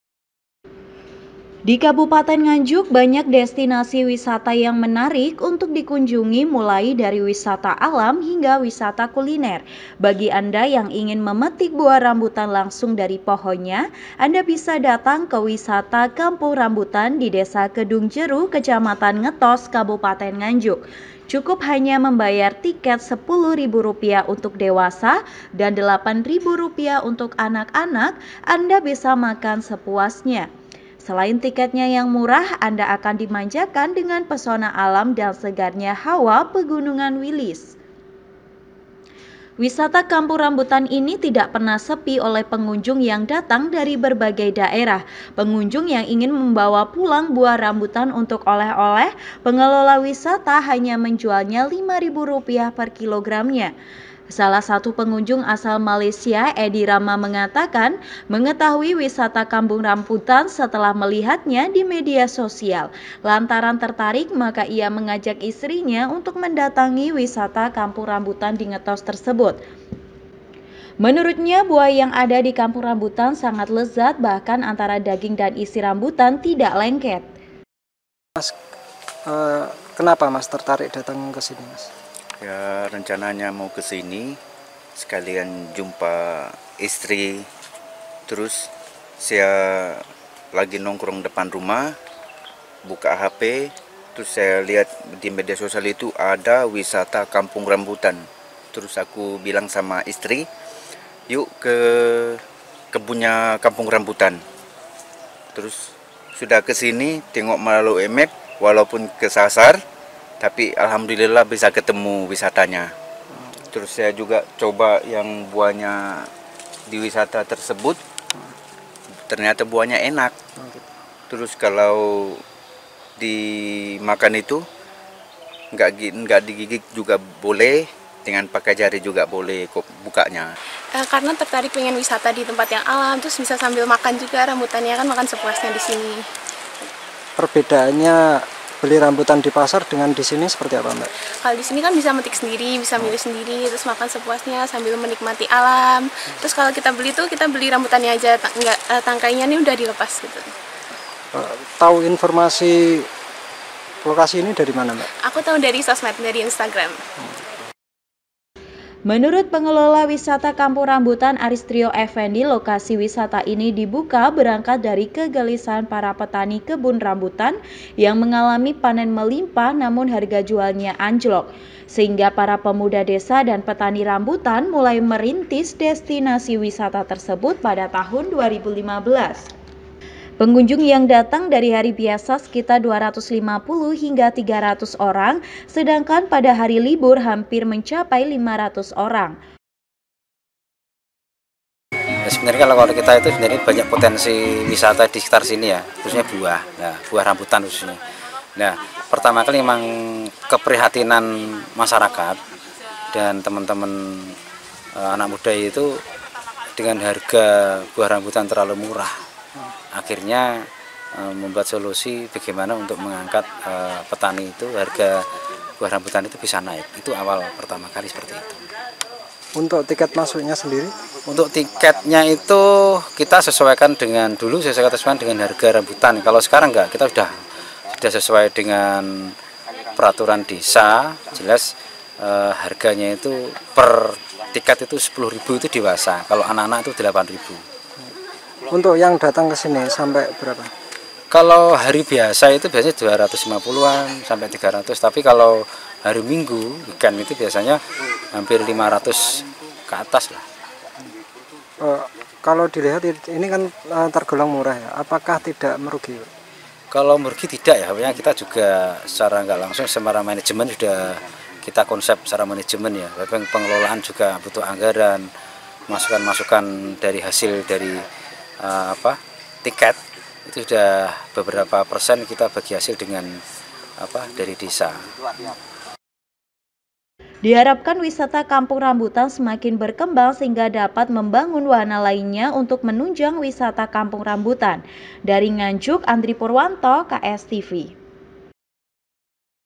NGANJUK - MENIKMATI WISATA PETIK BUAH DI KAMPUNG RAMBUTAN NGETOS NGANJUK
MENIKMATI WISATA PETIK BUAH DI KAMPUNG RAMBUTAN NGETOS NGANJUK